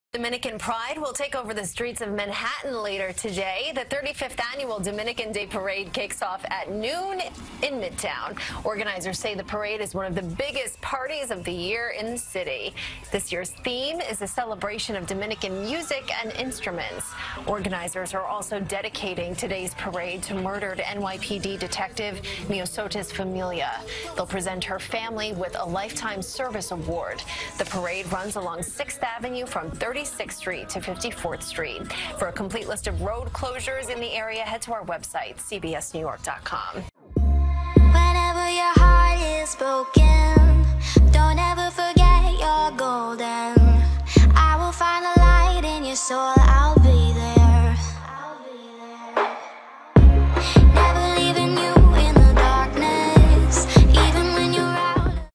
35th Annual Dominican Parade Taking Over Manhattan
The 35th annual Dominican Day Parade kicks off at noon in Midtown Sunday.

35th Annual Dominican Parade Taking Over Manhattan

35th Annual Dominican Parade Taking Over Manhattan